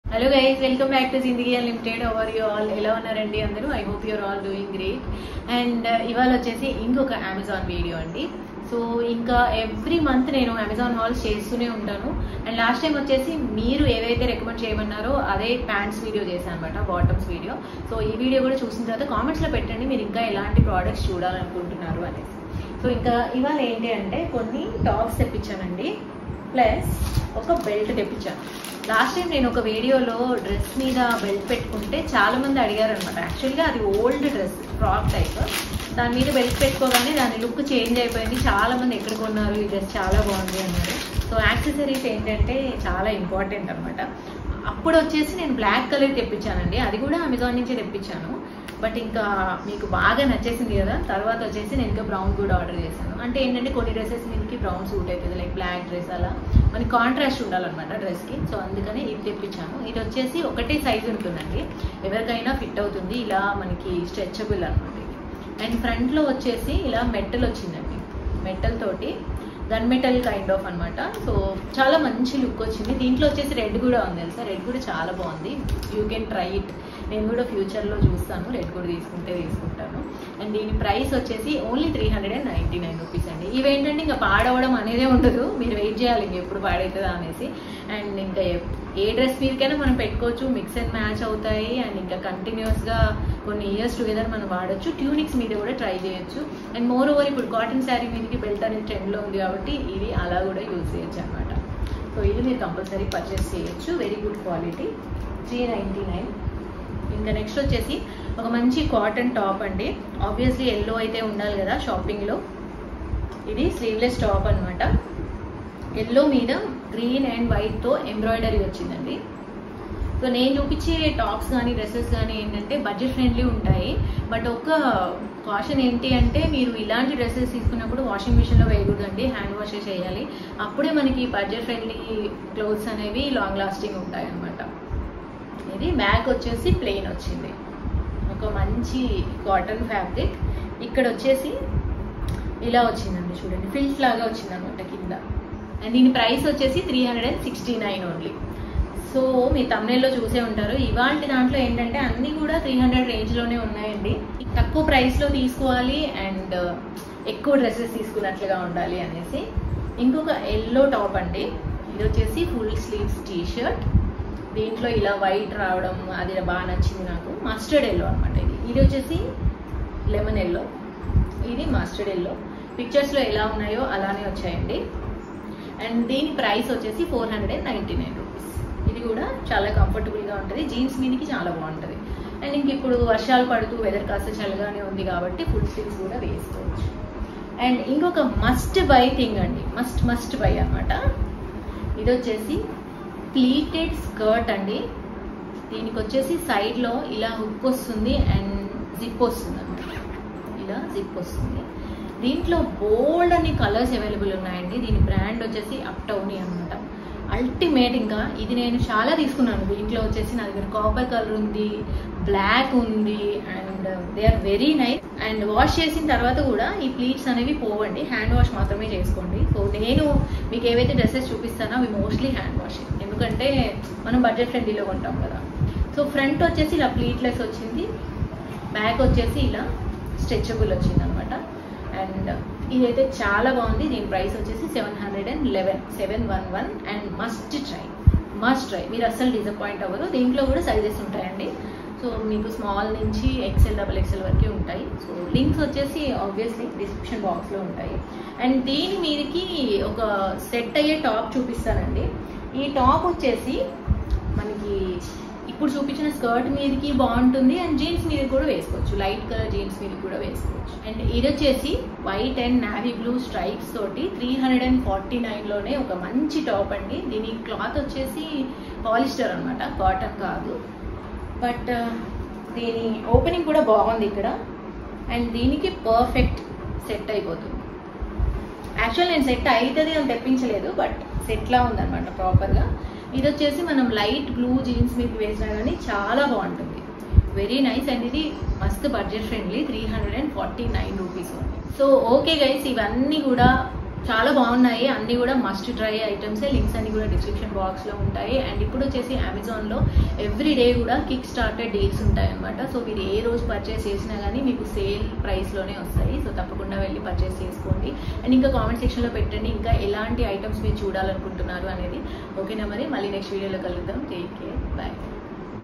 Amazon Shopping Haul: What I Bought and Why! Zindagi Unlimited Telugu Vlogs
అనుకోకుండా సెట్ అయింది || Amazon Shopping Haul || Zindagi Unlimited Telugu Vlogs

Buying Links :

1.  Pratibimb Women's Regular Creap Top (D-100_Blue M)

2.  AELOMART Women's Top (AMT4019) (Mustard, Large)

3.  Yash Gallery Women's Rayon Slub Embroidered Regular Top (Yellow)

4.  Uptownie Lite Women's Pleated Midi Skirt (Navy Blue,M)

5.  Electomania Women's Leather Belt (Camel color)

Watch Our Most Famous Videos here